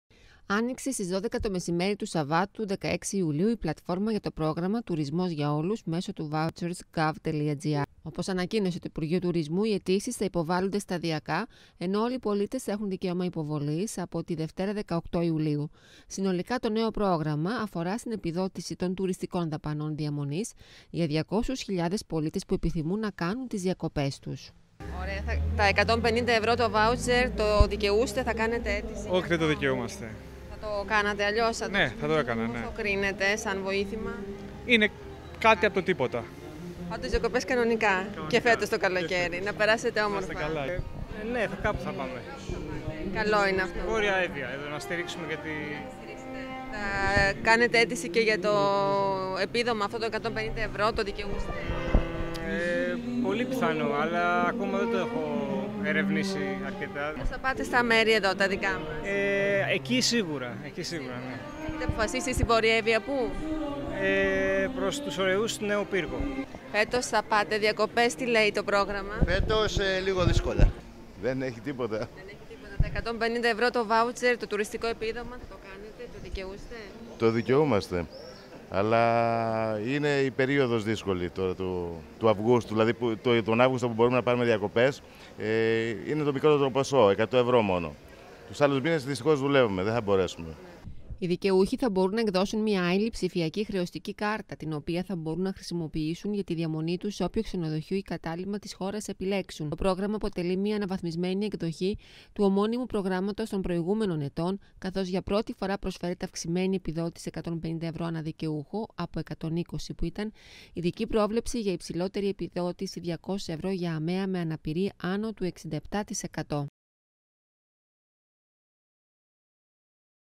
ΑΝΟΙΞΕ Η ΠΛΑΤΦΟΡΜΑ ΓΙΑ ΤΗΝ ΕΠΙΔΟΤΗΣΗ ΤΩΝ 150ΕΥΡΩ
Σε λειτουργία τέθηκε από σήμερα το νέο πλήρως αναβαθμισμένο πρόγραμμα «Τουρισμός για Όλους» που αφορά στην επιδότηση των 150 ευρώ σε τουριστικές δαπάνες διαμονής. Πολίτες που ρωτήθηκαν δεν το βλέπουν αρνητικά σαν κίνηση όμως δεν συμφωνούν με το ότι δεν το δικαιούνται όλοι παρά μόνο 200χιλιάδες.